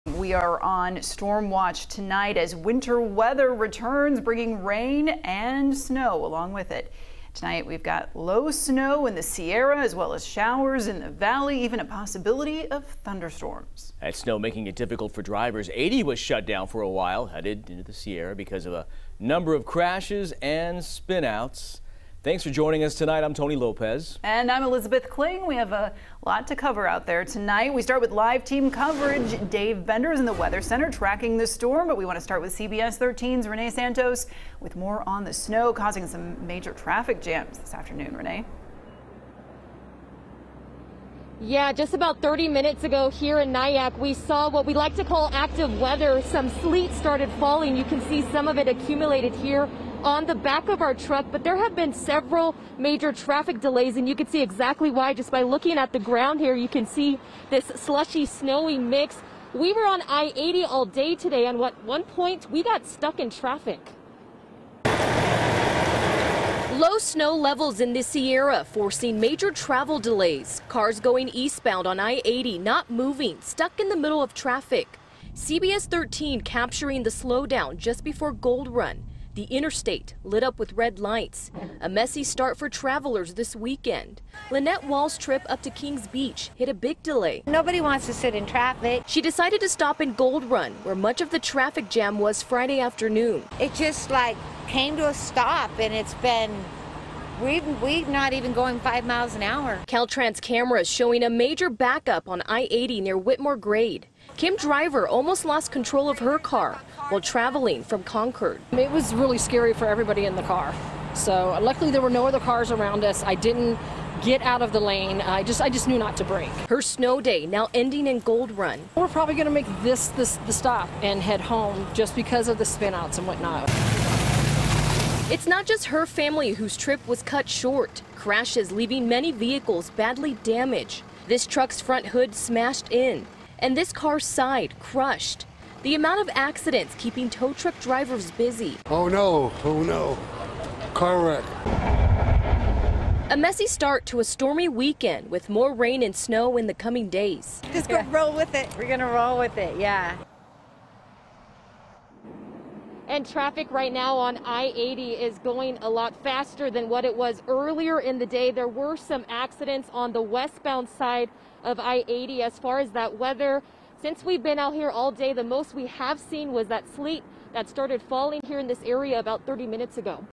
Snow Causes Traffic Mess On I-80
Low snow levels in the Sierra is creating major travel delays.